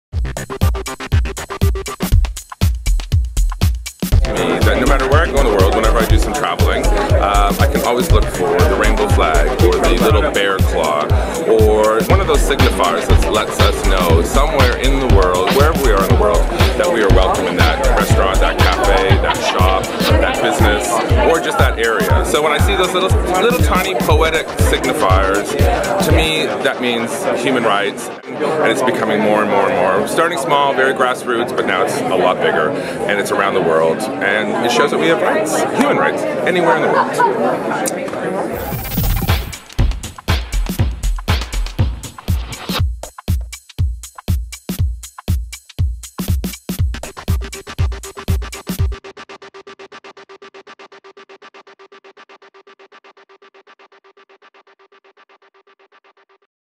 Keith Cole | WPHRC
What do global LGBT human rights mean to you?
Keith Cole | The WorldPride Human Rights Conference 2014